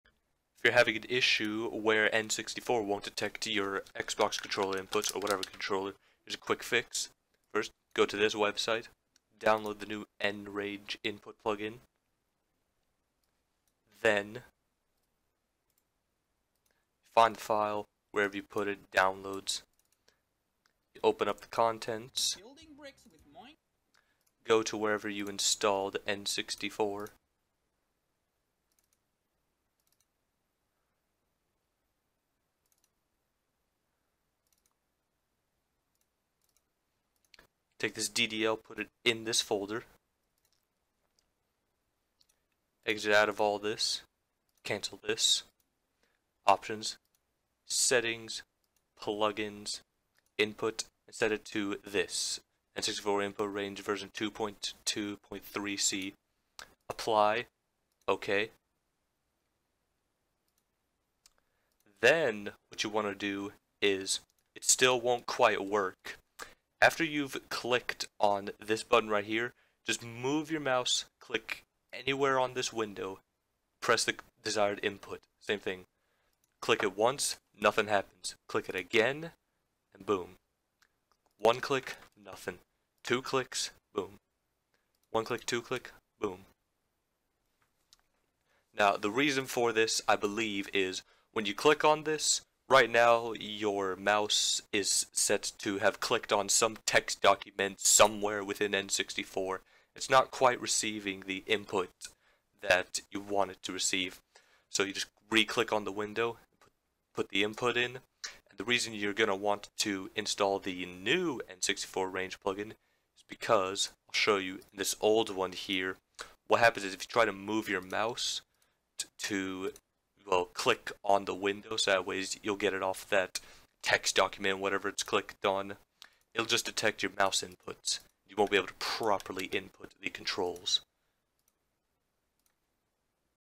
POSSIBLE Fix for Project64 Not Detecting Controller Input
Possible fix for Project64 not recognizing your controller input. This particular fix might not work for you, There could be MANY problems with Project64 not picking up on your controller. Here's ONE fix that nobody has talked about.